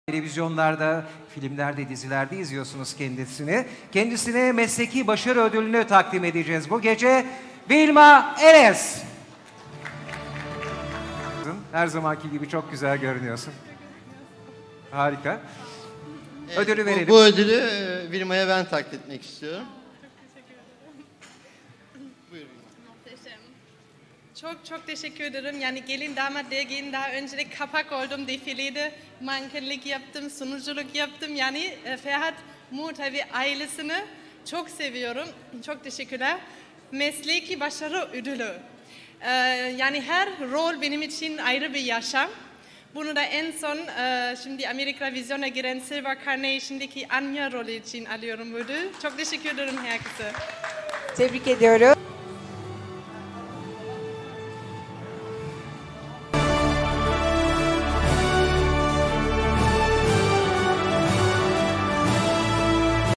Wilma Elles Röportaj - Gelin Damat Dergisi Ödülleri 2021
Wilma Elles,  Gelin Damat Dergisi bünyesinde imtiyaz sahibi Ferhat Muğurtay organizatörlüğünde düzenlenen Gelin Damat Dergisi Ödül Töreni'nin sunuculuğunu Pınar Altuğ Atacan ve Burak Törün yaptı.